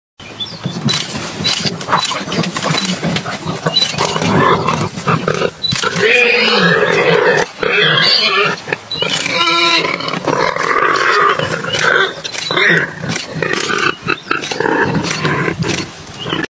I Ditched the Tourist Trail for This Leopard Attack — Here's Why
Leopard Attacks Warthog | Kruger National Park

Leopards are among the most skilled and elusive predators in Kruger National Park, and their interactions with prey, such as warthogs, are fascinating displays of natural behavior. When a leopard targets a warthog, it typically employs stealth and surprise to get close before launching a swift and powerful attack. The leopard's agility and strength allow it to overpower the warthog, often aiming for a quick kill by targeting the neck or throat.

Encounters between leopards and warthogs are thrilling to witness, showcasing the raw and untamed dynamics of the African wilderness. These moments are a testament to the incredible adaptability and hunting prowess of the leopard, one of Kruger National Park's most iconic and formidable predators.